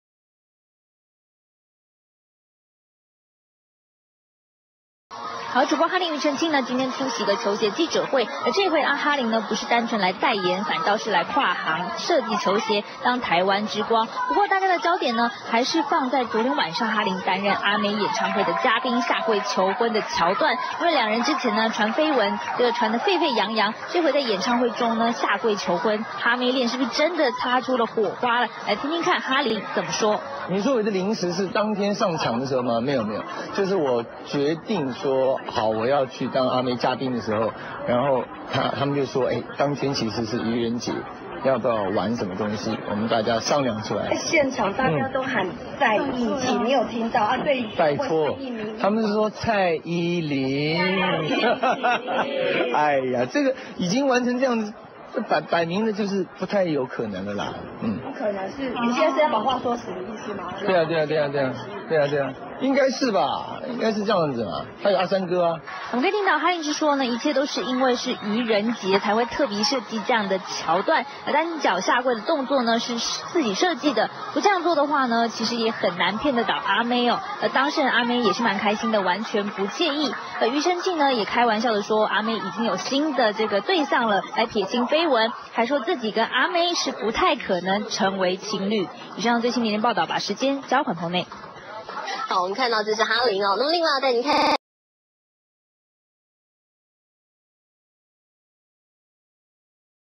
KruZIn Creative Director Alessandra Gold and the Harlem Yu collaboration launch
KruZIn Creative Director Alessandra Gold and the Harlem Yu collaboration launch in Taipei, Taiwan - April 2, 2012